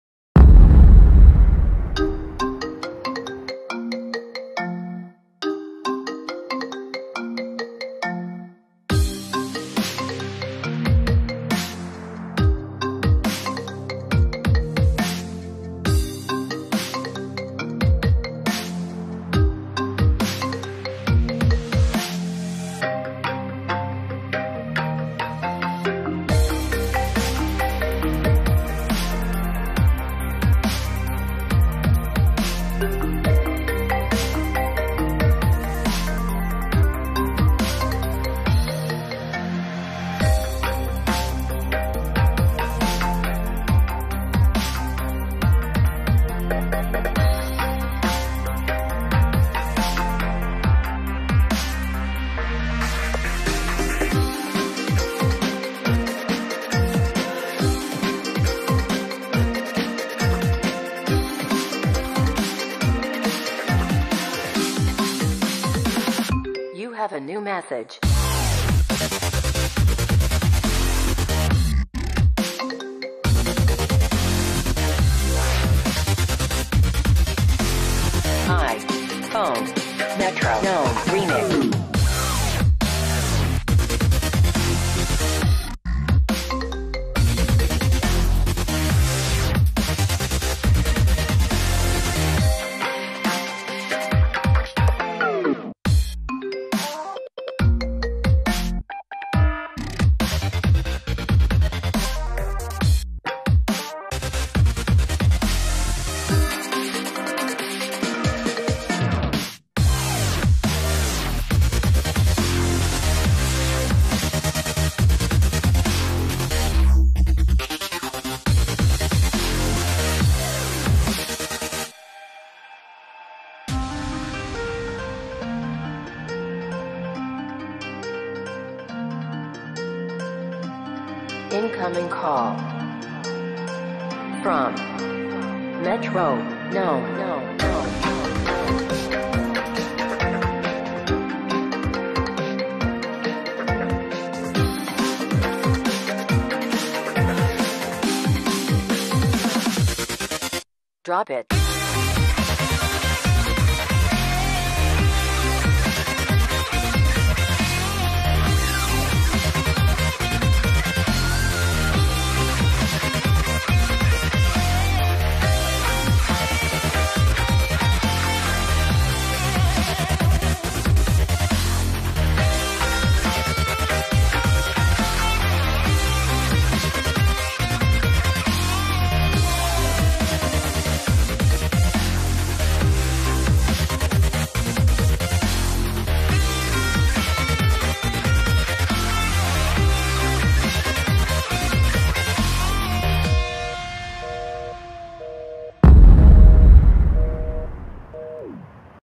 GoPro Climbing Indoor Test (RockSpot)
prova degli accessori gopro della marca smatree, in rockspot 2 a pero, milano. timelapse della zona boulder.
tutte le riprese sono state fatte con la GoPro Hero 3 White Edition